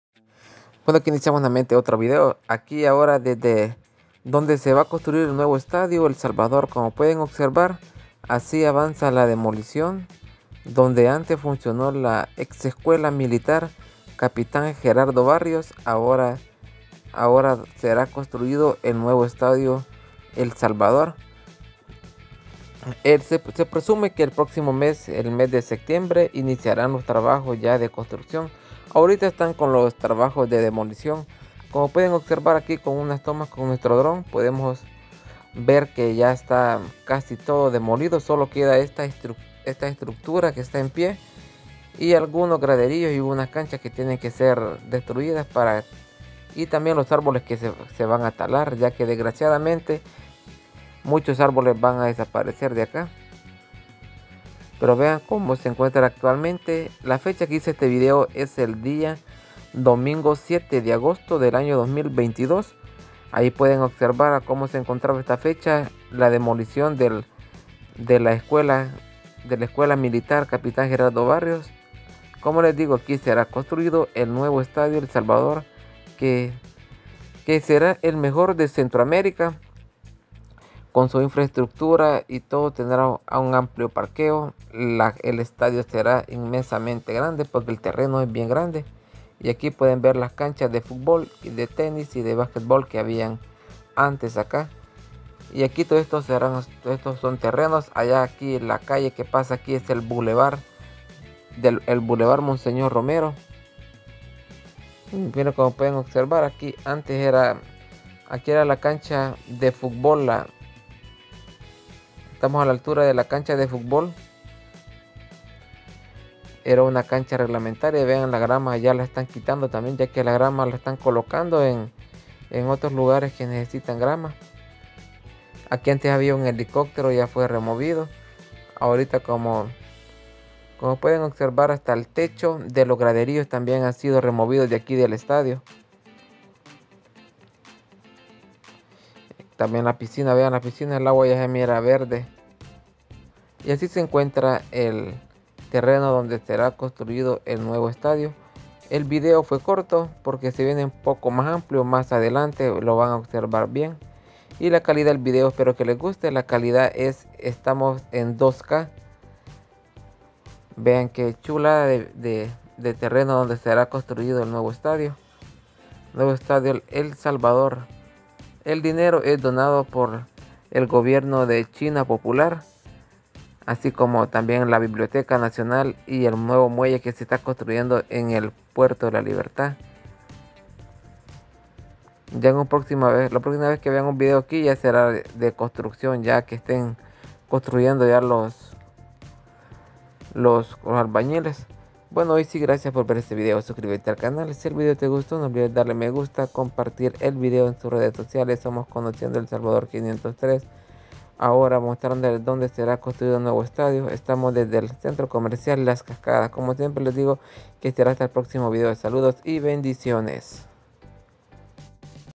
NUEVO ESTADIO EL SALVADOR ( avance de demolicion )
hoy iniciamos nuestro vuelo donse se construira el nuevo estadio el salvador, aqui mostramos como avansa la demolicion de la escuela militar.
Javier Galdamez.